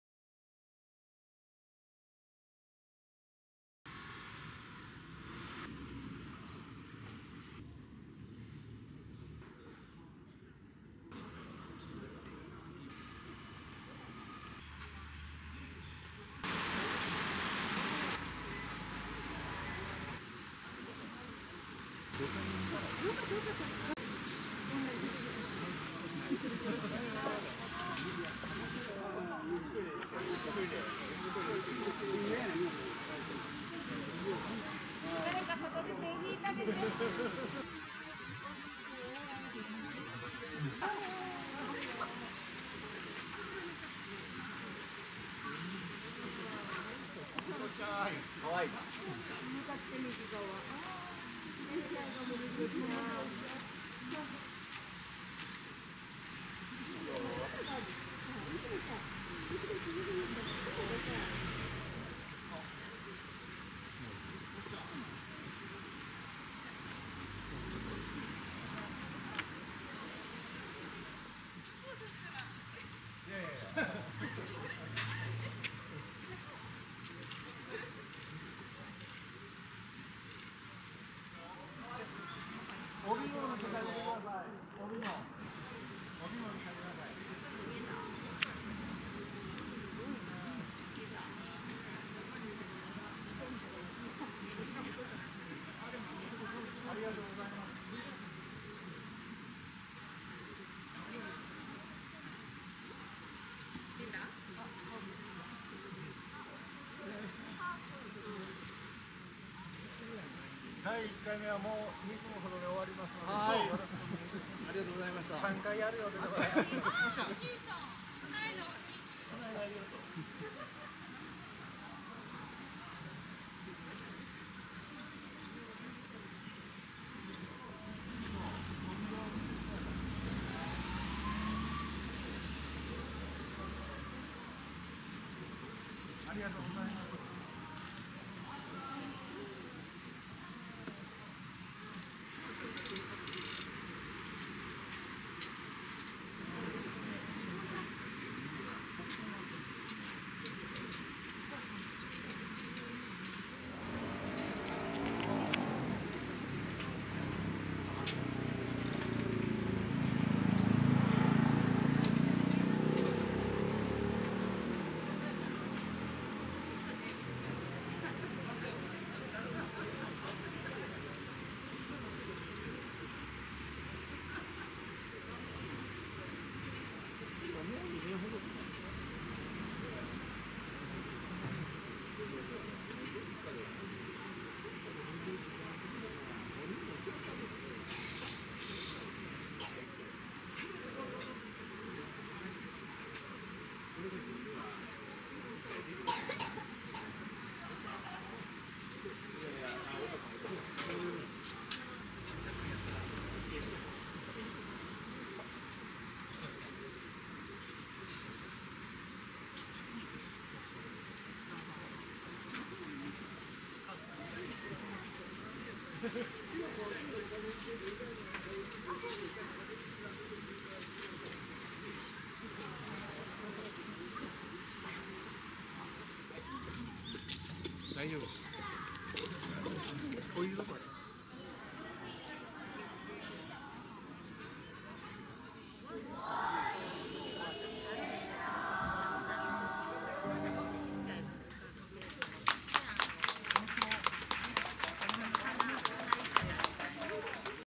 京都 高瀬川 舟まつり 舞妓さんの舟上撮影・お茶の接待（2023/10/15）
Maiko photo shoot and tea ceremony at Takasegawa River in Kyoto